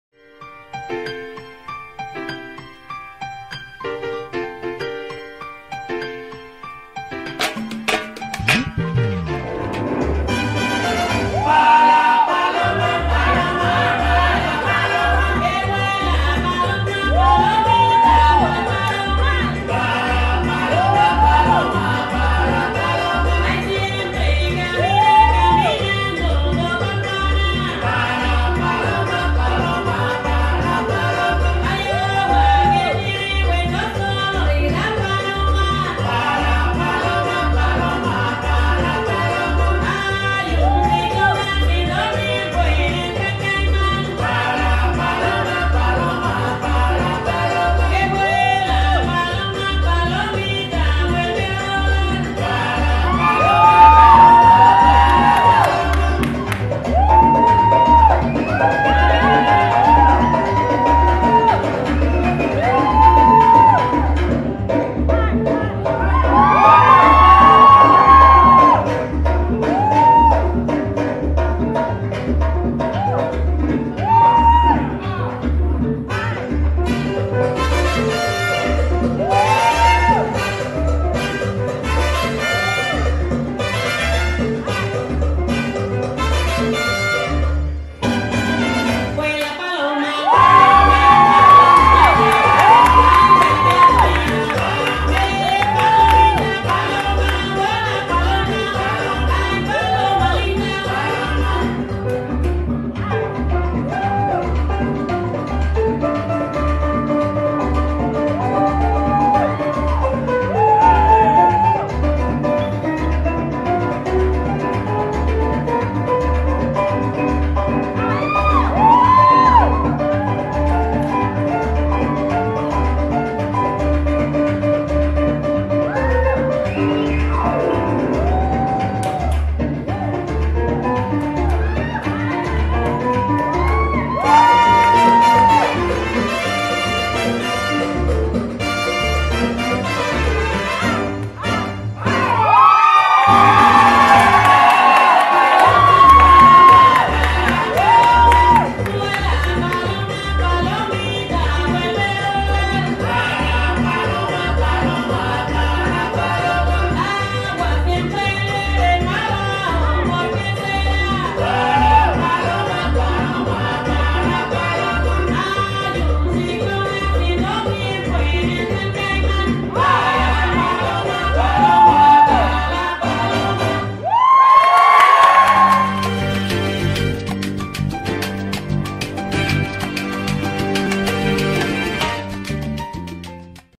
P'Kante Dance Company - Las Chicas Locas - LatinParty.com
P'Kante Dance Company performing at Las Chicas Locas at DanceSport 22 West 24th Street, New York